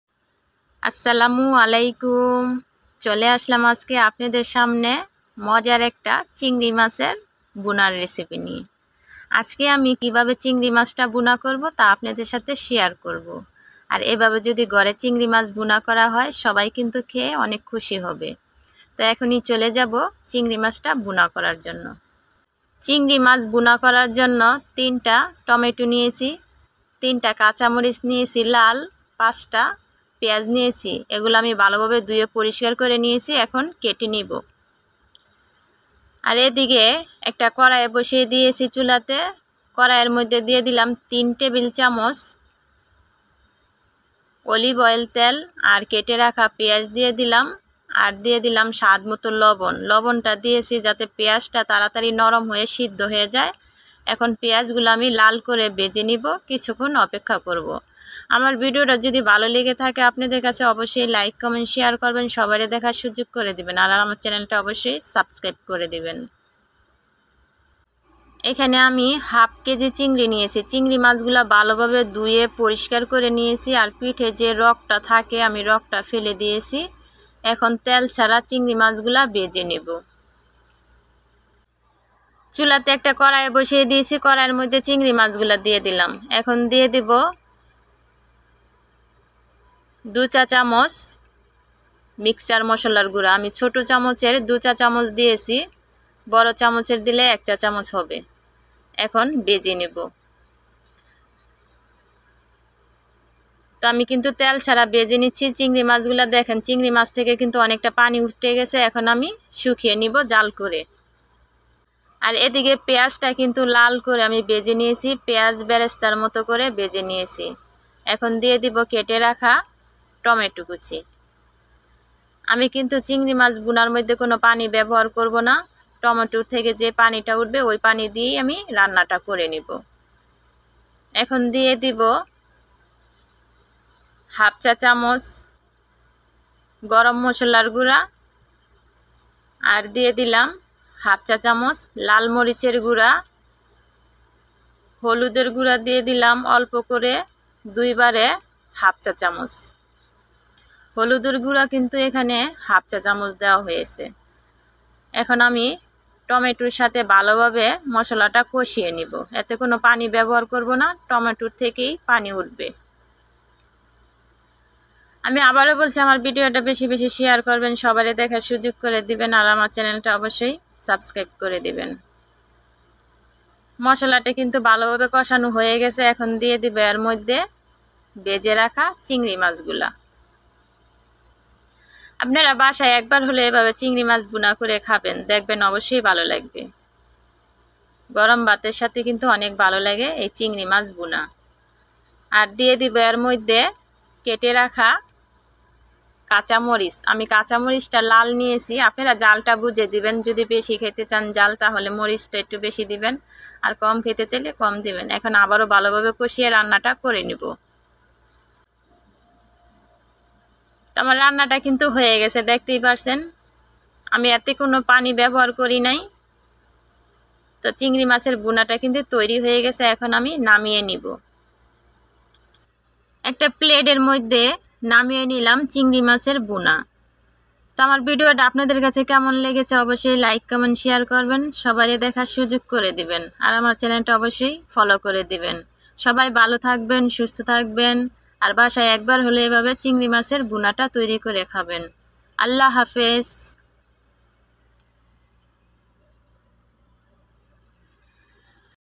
টমেটো দিয়ে চিংড়ি মাছের ভুনা।
এই ভাবে যদি টমেটো দিয়ে চিংড়ি মাছের ভুনা করা যায় চোখের পলকে শেষ করে দিবে প্লেটের ভাত।

হাফ কেজি  চিংড়ি মাছ। ভালো ভাবে ধুয়ে পরিস্কার করে  ভেজে নিয়েছি তেল ছাড়া।

তিন টেবিল চামচ  অলিভ তেল। তিনটি  টমেটো, ৫ টি পেঁয়াজ, তিনটি  লাল কাচ মরিচ।

লবন পরিমান মত, হাফ চা চামচ গরম মসলার গুড়ো, হাফ চা চামচ মরিচের গুঁড়ো, হাফ চা চামচ হলুদের গুড়ো।

এই উপকরণ গুলো লেগেছে আমার  চিংড়ি মাছের ভুনা রান্না  করতে।